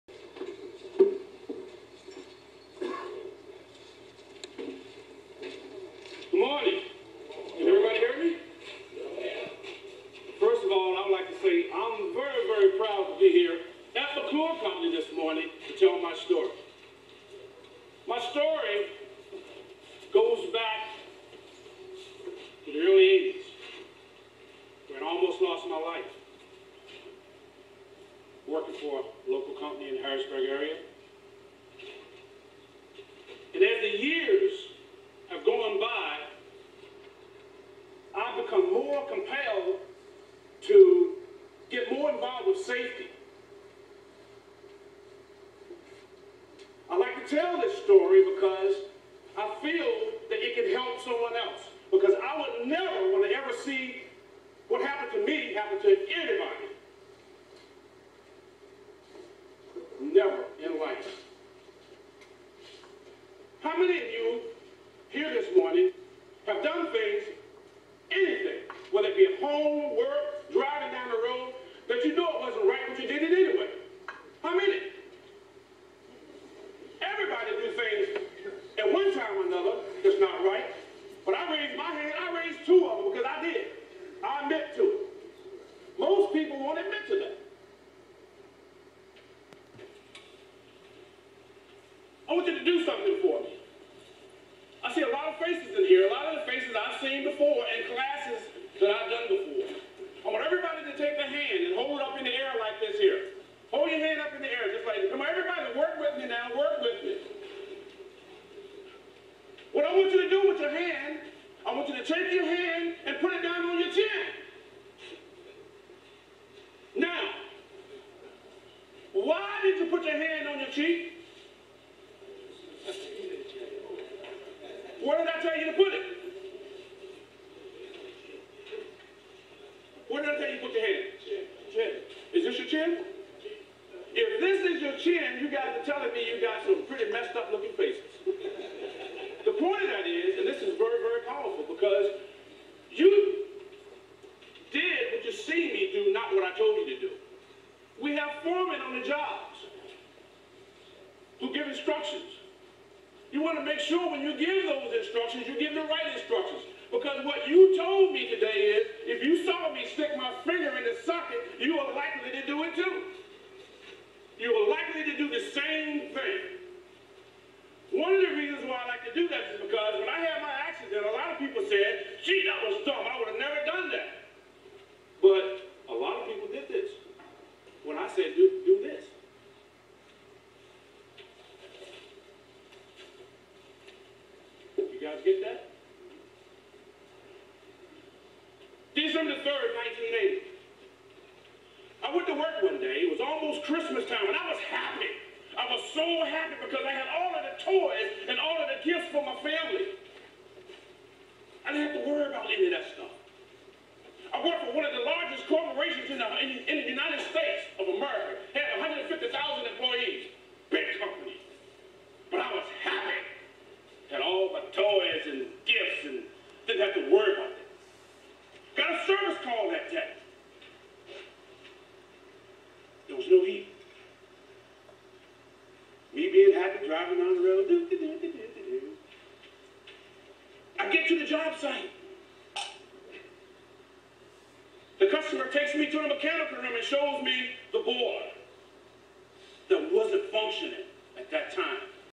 Ron Goodrich safety lecture
Ron's personal account of an accident at the work place that almost cost him his life.